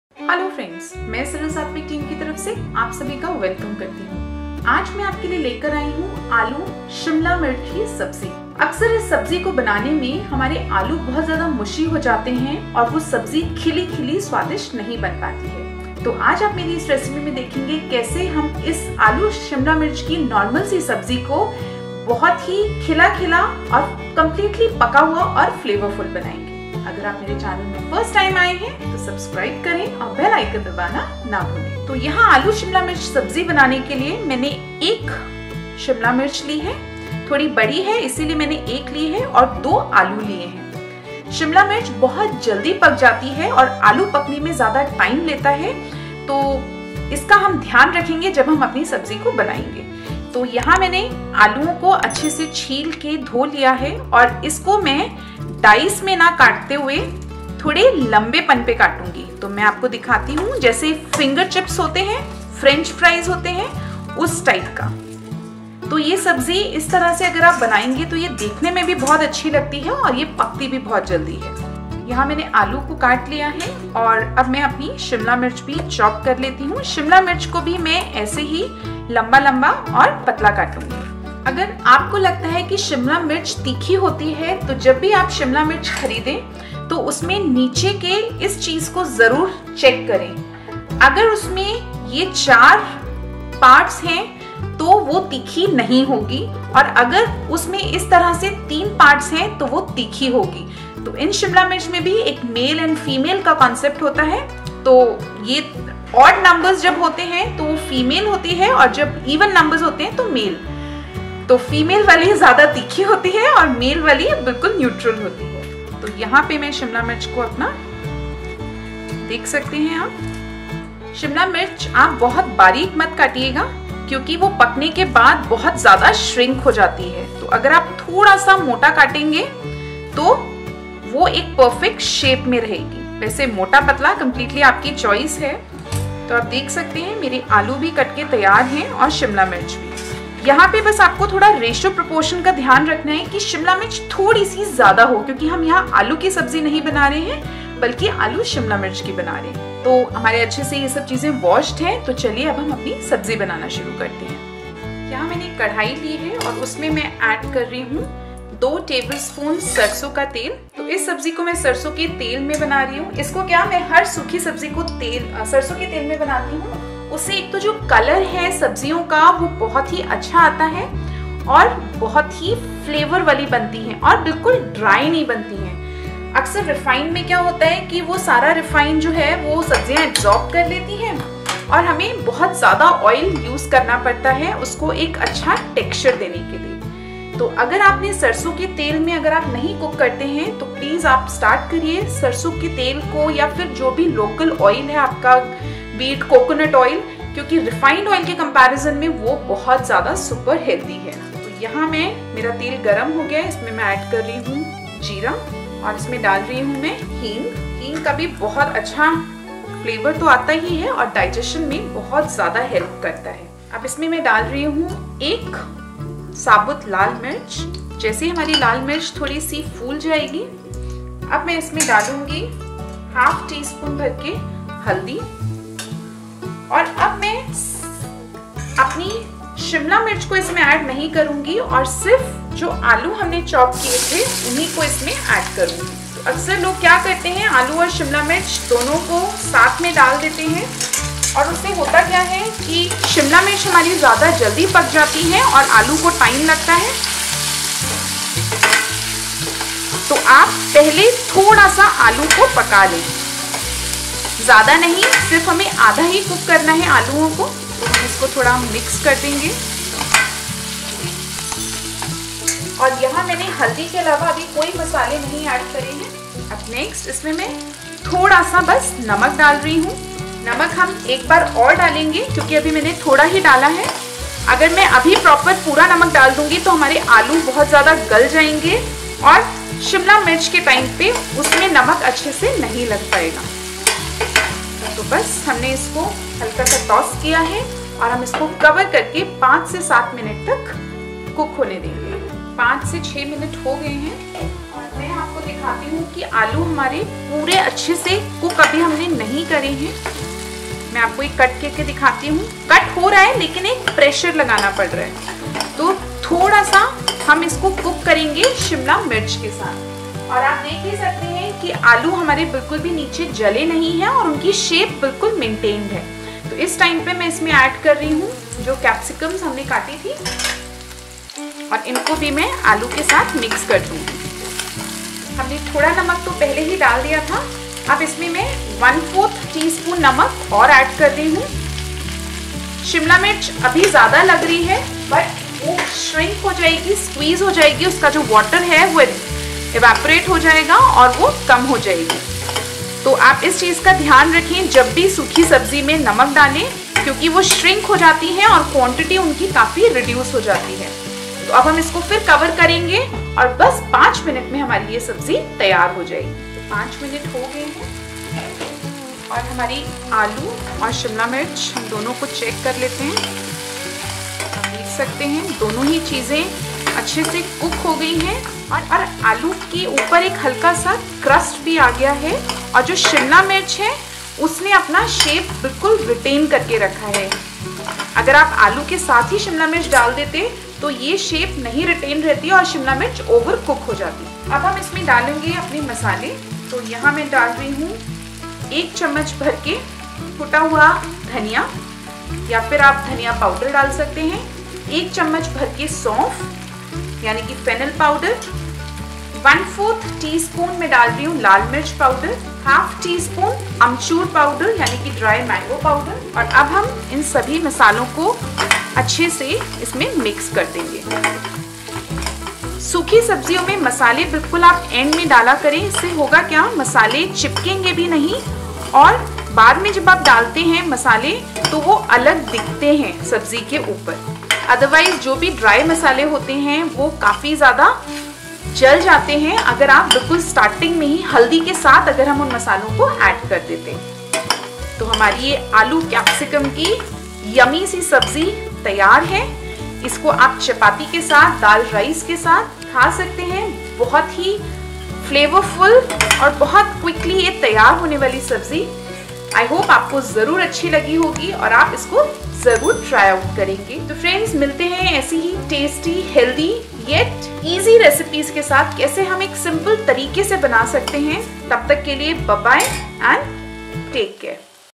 Aloo Shimla Mirch Sabji (Capsicum Potato) | आलू शिमला मिर्च सब्जी को खिला खिला कैसे बनाएं
SaralSatvik From Saral Satvik we present for you this recipe of Aloo Shimla Mirch Sabji or Capsicum Potato Sabzi. If you have been cooking this recipe earlier you might have observed the meshy texture of this sabji where Bell Pepper is over cooked often. This makes it less attractive and lesser tasty.
In this recipe we focus on this main issue and try to give you the awesome taste of Capsicum as it is.

Special taste and health tips during the recipe video.